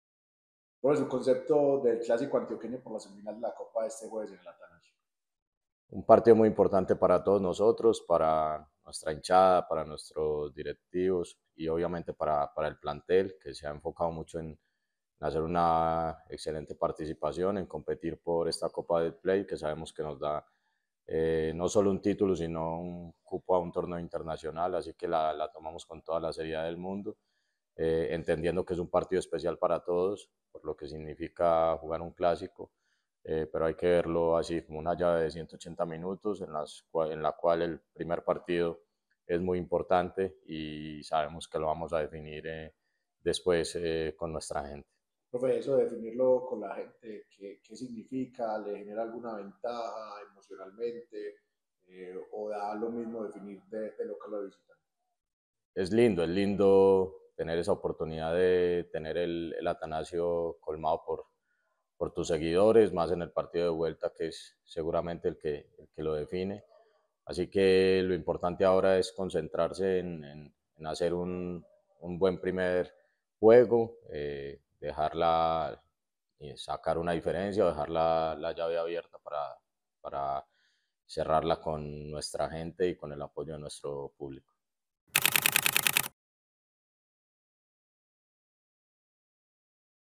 [Reacciones DIM 🎙️🔴🔵] Alejandro Restrepo en la previa a la semifinal de ida
El Poderoso enfrentará a su rival de patio este jueves por la ida de la semifinal de la Copa Betplay Dimayor 2024. El entrenador Alejandro Restrepo habló con el medio oficial previo a dicho encuentro.